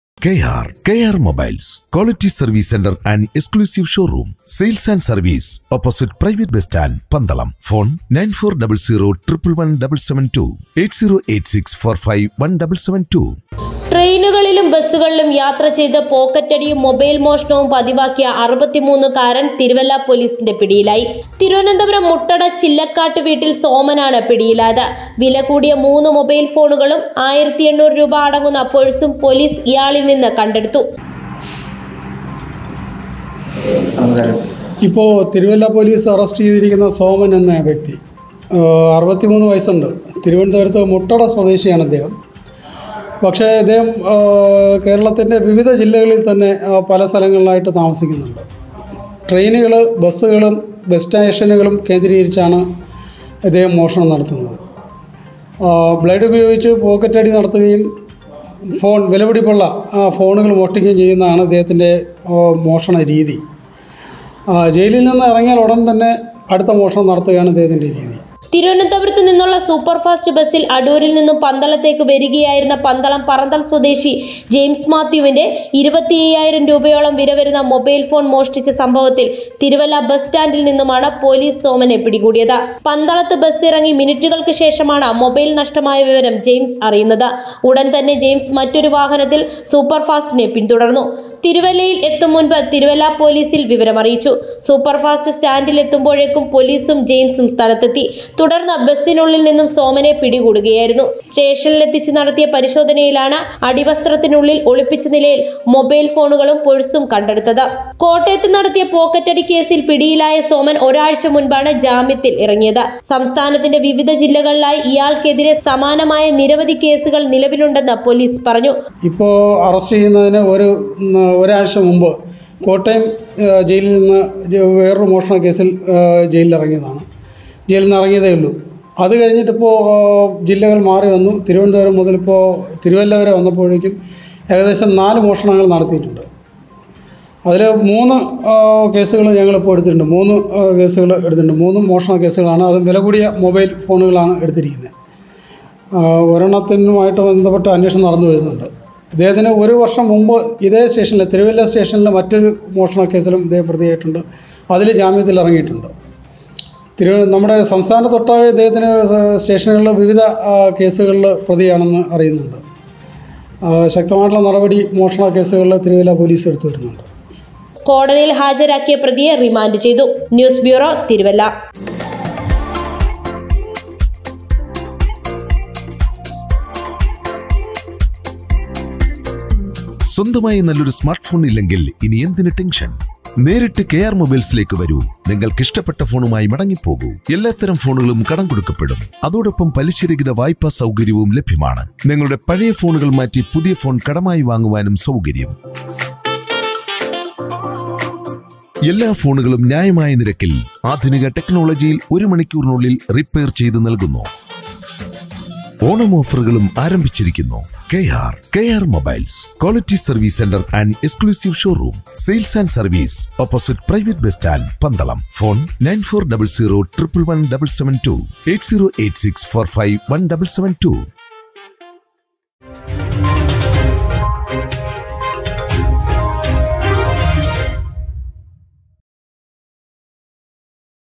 പോക്കറ്റടിയും മോഷണവും പതിവാക്കിയ 63 കാരൻ പിടിയിലായി
തിരുവനന്തപുരം മുട്ടട ചില്ലക്കാട്ട് വീട്ടിൽ സോമൻ ആണ് പിടിയിലായത്
തിരുവല്ല ബസ് സ്റ്റാൻ്റിൽ നിന്നാണ് പോലീസ് പിടികൂടിയത്